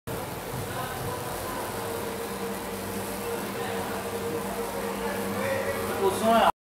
Poste apresenta problemas na fiação
Um poste na Rua Vigário Tenório emite barulho e faíscas